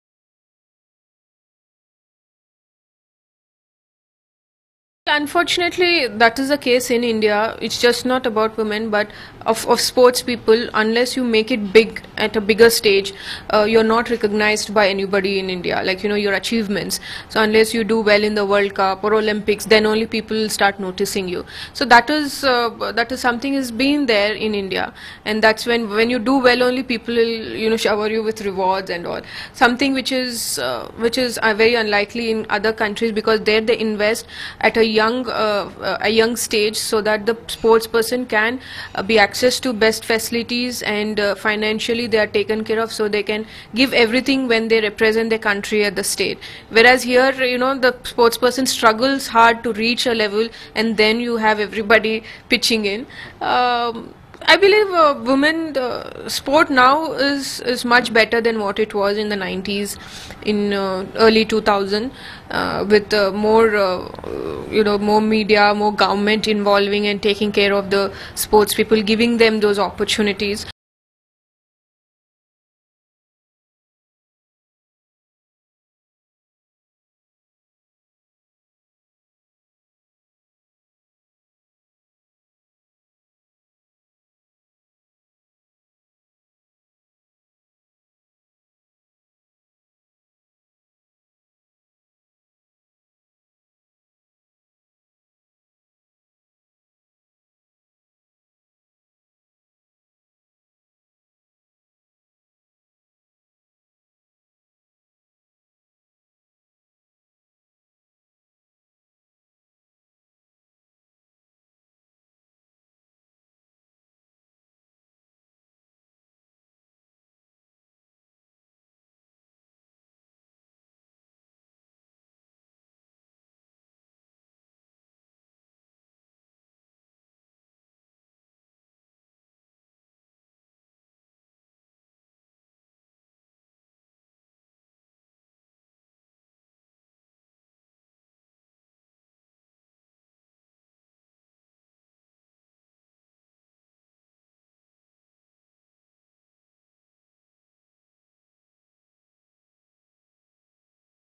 Unfortunately, sportspersons are not recognized in India unless they make it big: Mithali Raj
Indian Women’s Cricket Team Captain Mithali Raj said it is unfortunate that sportspersons are not supported in India unless they make it big.